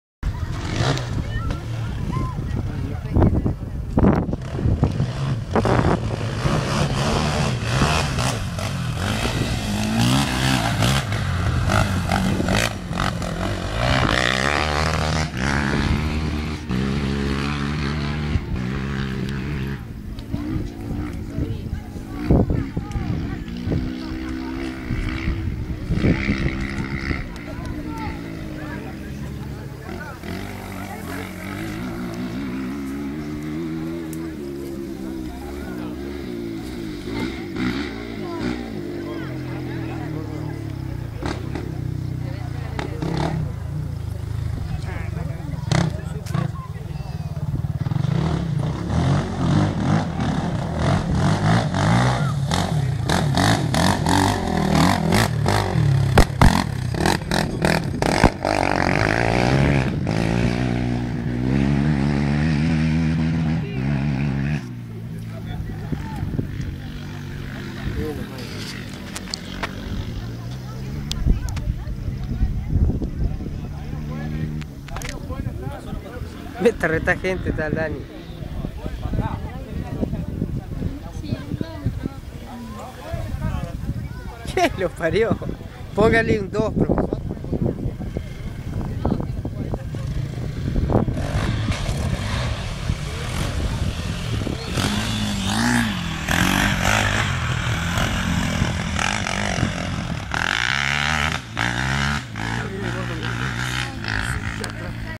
CUATRICICLOS EN GUICHON
PASADA POR ARROYO DE LOS CUATRICICLOS, ESTO FUE EN GUICHON, EL MEJOR CIRCUITO ENDURO DEL PAIS